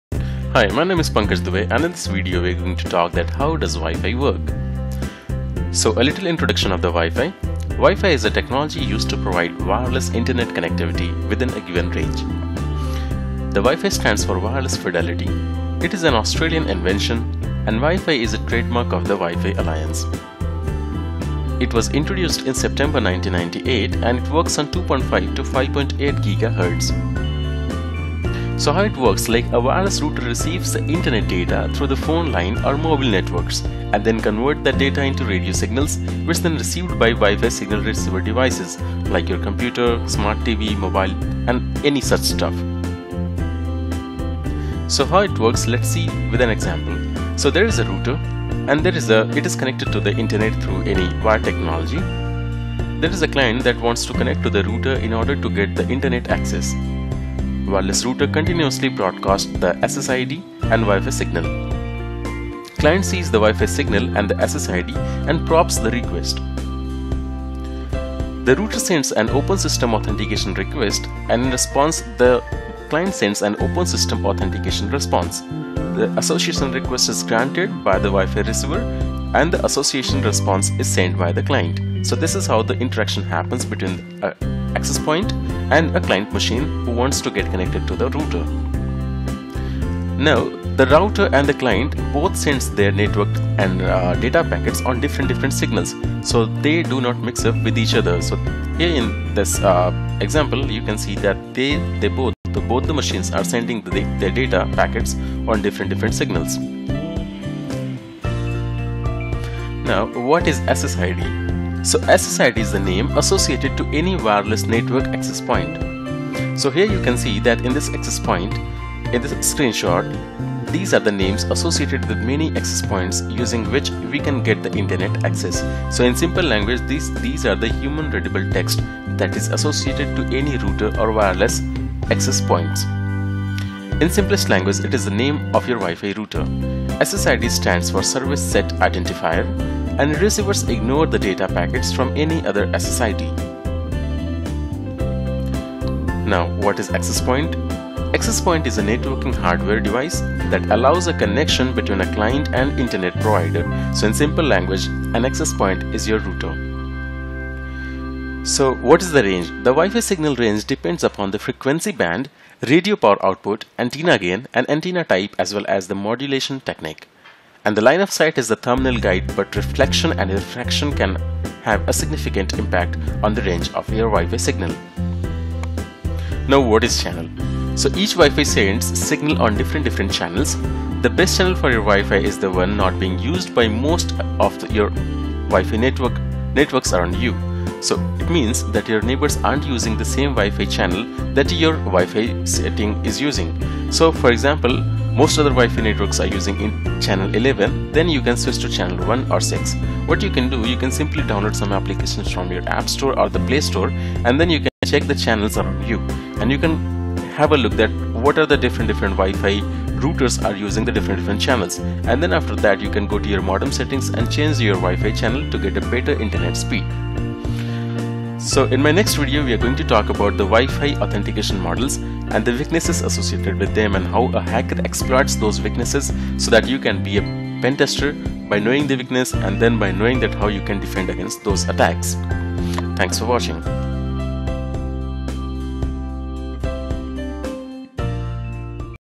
How wifi works
In this video, you will learn how wireless networking works and the basics of wifi.